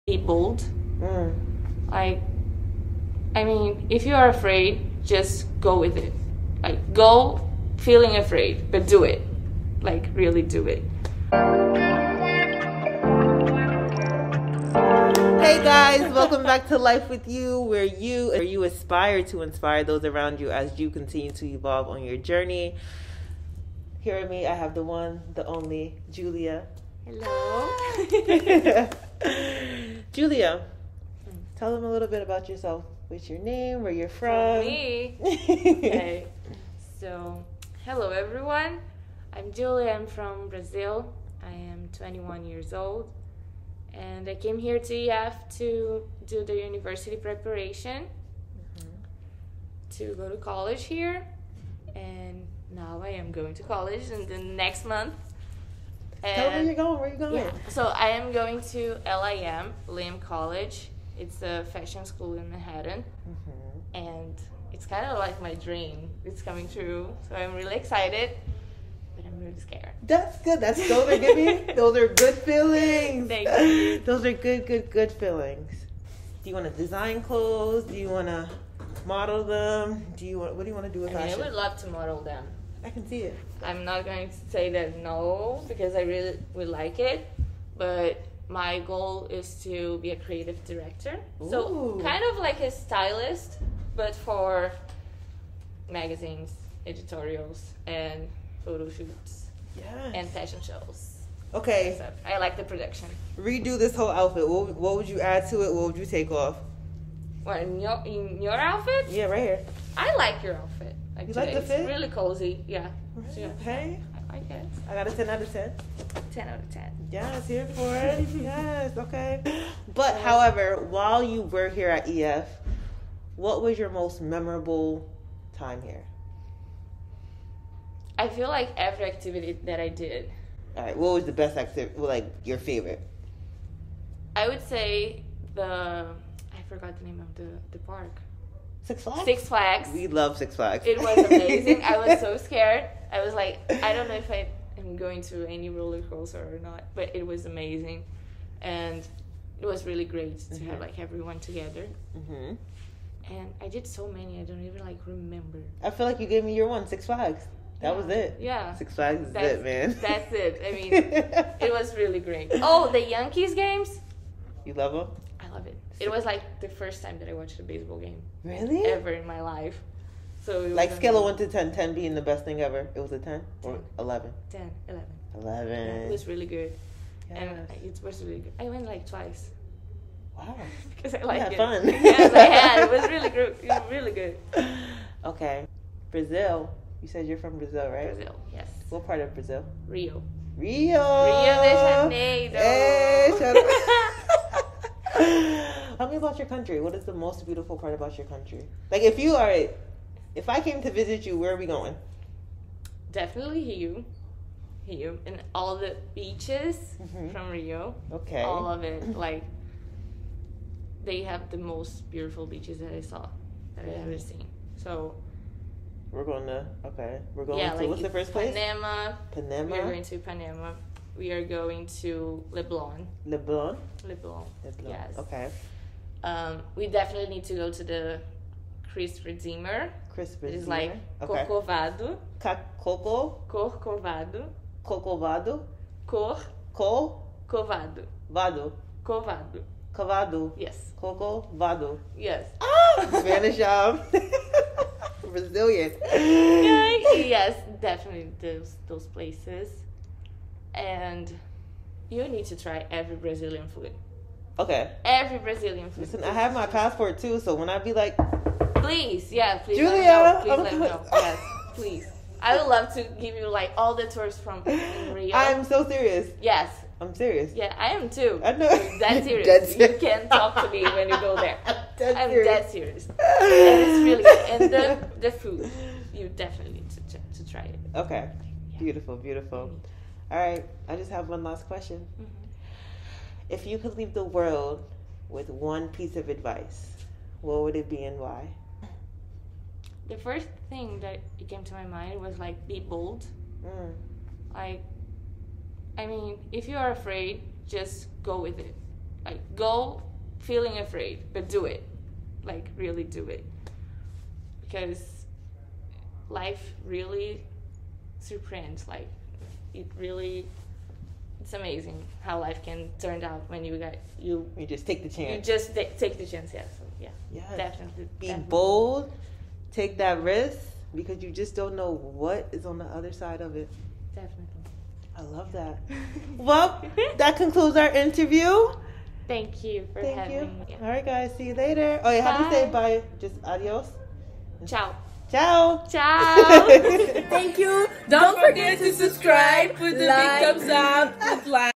EF Education First: Julia Mattos
“Be bold. If you’re afraid, just go with it. Go feeling afraid but do it because life is amazing when you just take the chance.” -Julia Mattos

Tune in and listen to EF Student Julia as she shares her experience here at EF, what she’s going to school for, some of her favorite things about Brazil  and one piece of advice for the world before her departure back home to Brazil.